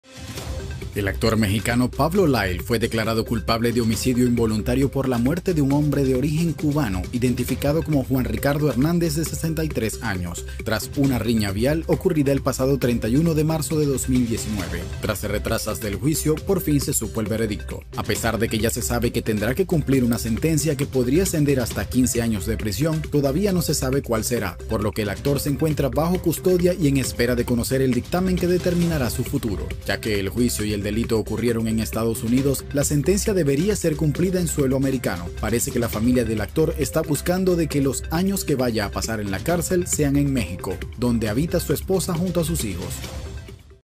El actor Pablo Lyle podría cumplir condena en México y pasar hasta 15 años de cárcel
El actor Pablo Lyle fue declarado culpable y dicen que podría pasar hasta 15 años en la cárcel. Pero dicen que podría cumplir condena en México.

hombre de origen cubano identificado como Juan Ricardo Hernández, de 63 años, tras una riña vial ocurrida el pasado 31 de marzo de 2019. Tras retrasas del juicio, por fin se supo el veredicto.

A pesar de que ya se sabe que tendrá que cumplir una sentencia, que podría ascender hasta 15 años de prisión, todavía no se sabe cuál será, por lo que el actor se encuentra bajo custodia y en espera de conocer el dictamen que determinará su futuro.

Ya que el juicio y el delito ocurrieron en Estados Unidos, la sentencia debería ser cumplida en suelo americano. Parece que la familia del actor está buscando que que los años que vaya a pasar en la cárcel sean en México, dónde habita su esposa junto a sus hijos.